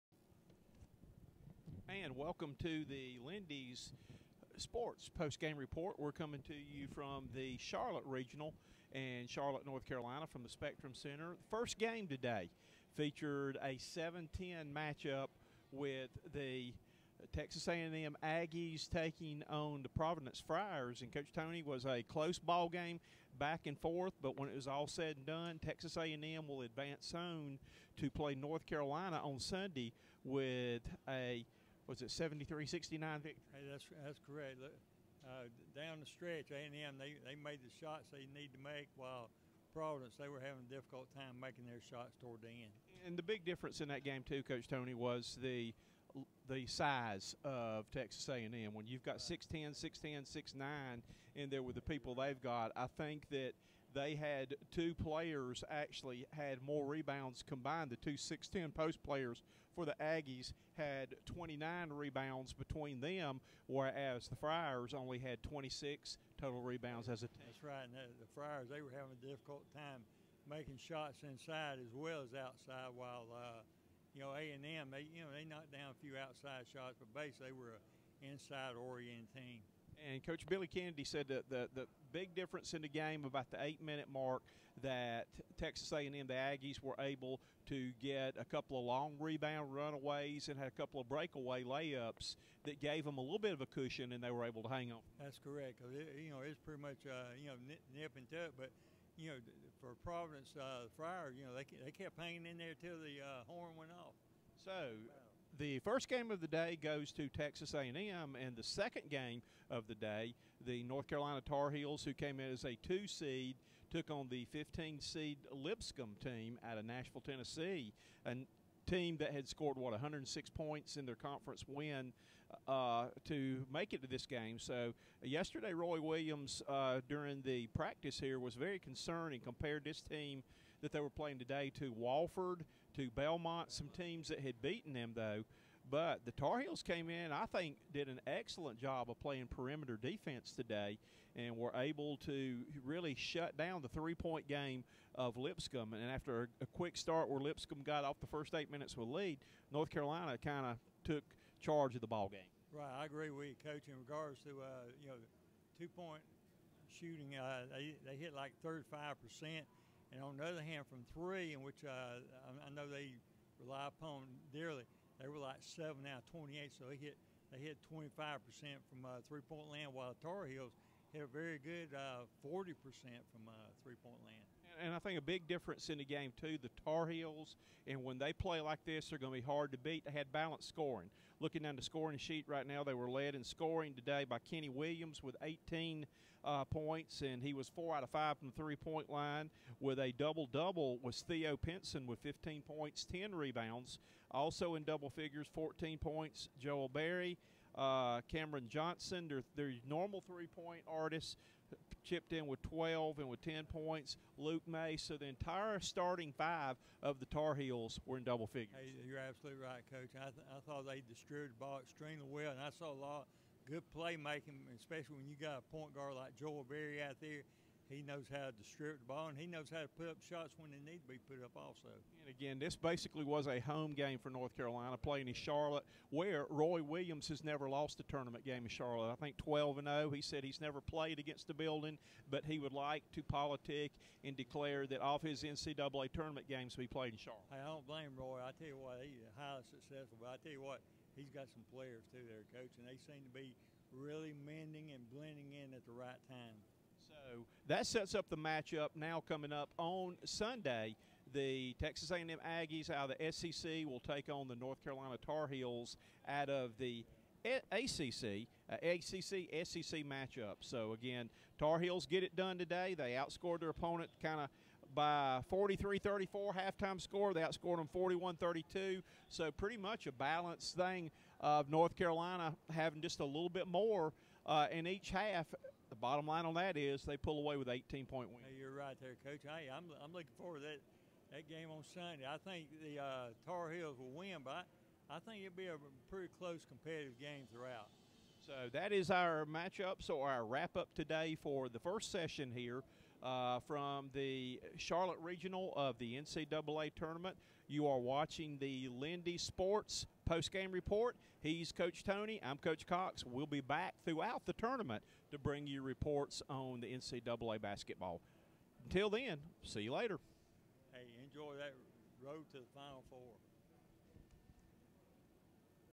Lindy’s Sports Post Game Report: NCAA Charlotte Regional
Dick Cox and Barry Toney report on the Texas A&M victor over Providence and the North Carolina Tarheels win over Lipscomb in the first season of the Charlotte NCAA basketball regional